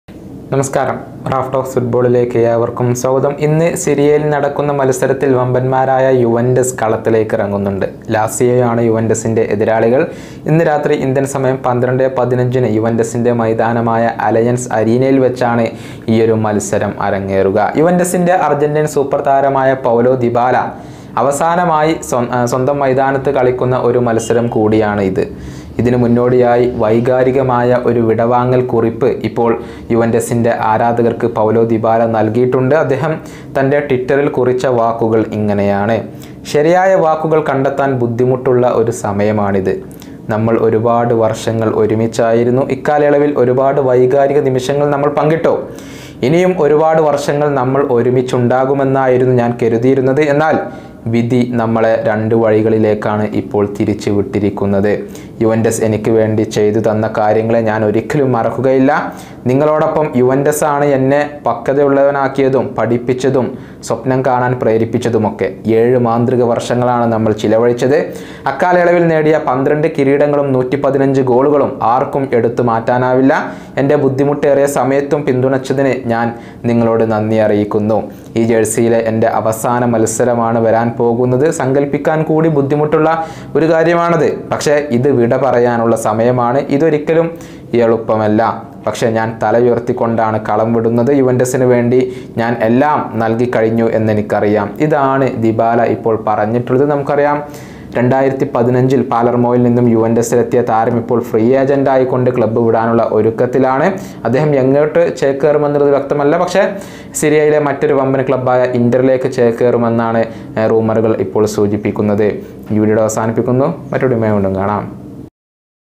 ഒരുപാട് കാലം ഒരുമിച്ചുണ്ടാവുമെന്ന് കരുതി: വിടവാങ്ങൽ സന്ദേശവുമായി ഡിബാല! | Transfer News | Dybala
Paulo Dybala
Juventus